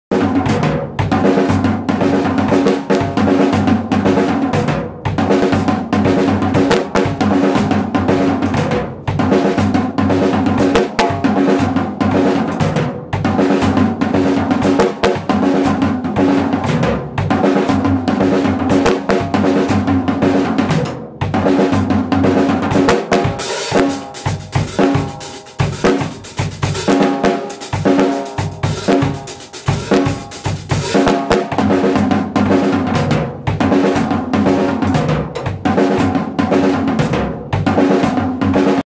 Playing a Heavy Tom-Tom Drum Groove | YoungJesseBee
I don't always use a lot of tom-toms. You might say that I have a love/hate relationship with them. When they sound good, they sound SO good and nothing else will do, but when they sound bad they are just so muddy and dull-sounding. A bad tom-tom can really ruin a song.

Toms are fun to play though and provide some nice dramatic sounds that are essential to certain songs and styles. That being said, they have to be tuned correctly. A high-tuned tom-tom sounds more like a timbale. Generally, when we think of "thundering" tom-toms, we are thinking of very low-tuned drums, sometimes so low that you can hear the drumheads flapping as you hit them.

I'll keep my toms around, but I'll be mindful that sometimes they aren't needed, and other times they're just the thing.